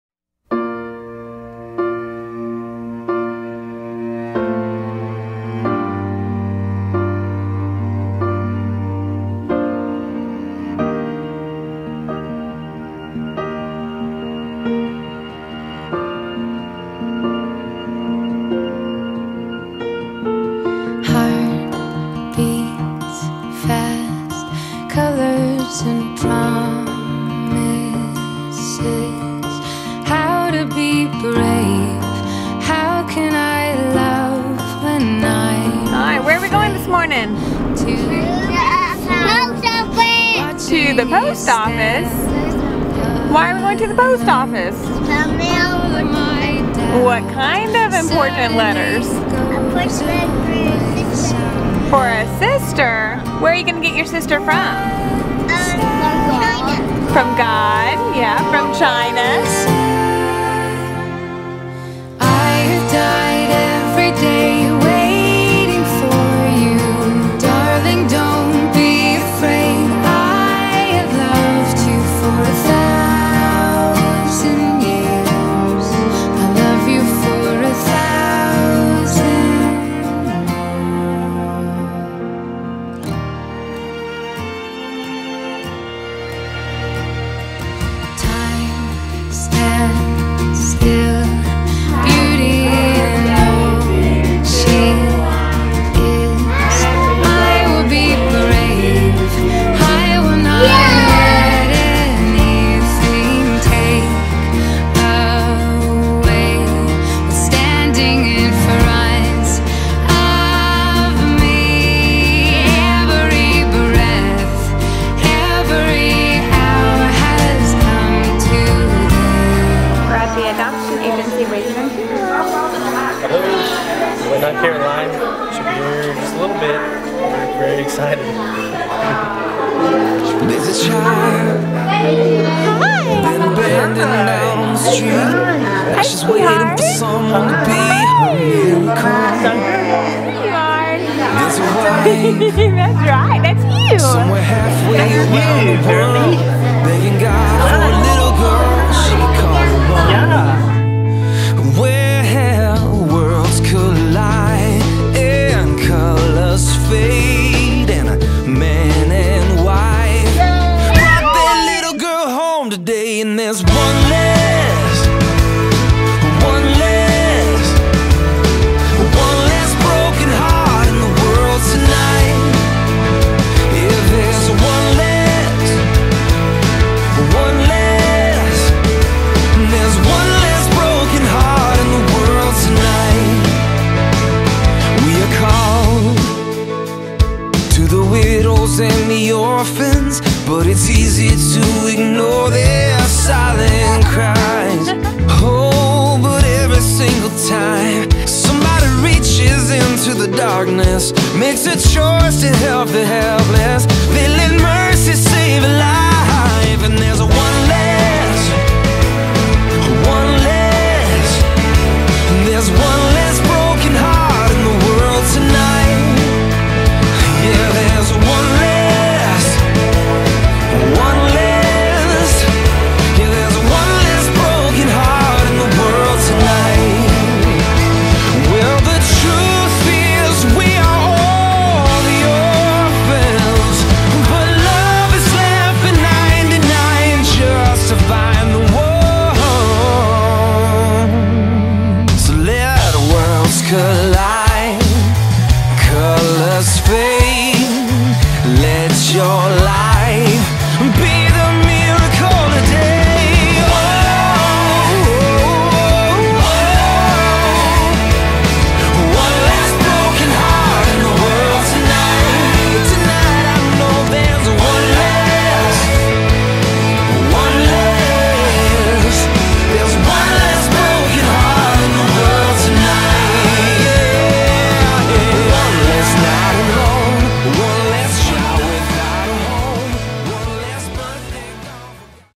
Our Adoption Journey to Caroline Guangdong China.
Our little girl adopted from Guangzhou China in 2015.  This is her story of finding a family and our story to find our daughter.